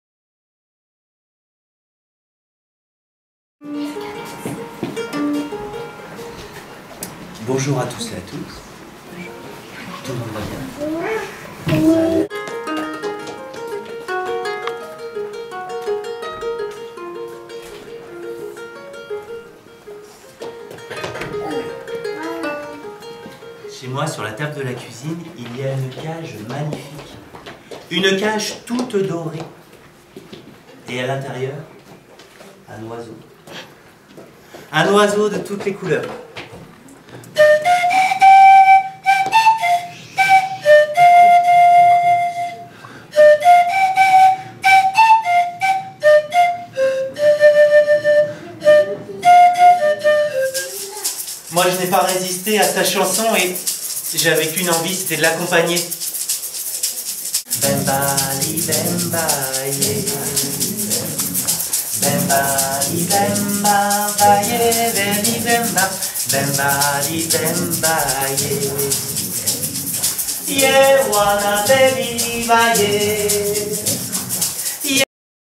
Le chant de la liberté
Voici quelques extraits du spectacle "Le chant de la liberté" captés par Vincent lors de la représentation du 31 janvier 2016 à La Roche Sur Yon (85) dans le cadre du festival In-Ouïe.
Ce spectacle est une des création de Eric Mandret pour la compagnie Luciole et Grillon. C'est un moment à partager de 6 à 1066 mois. Créé en crèche puis en école maternelle en 2015, ce spectacle permet au public de faire un voyage sonore en compagnie d'un oiseau.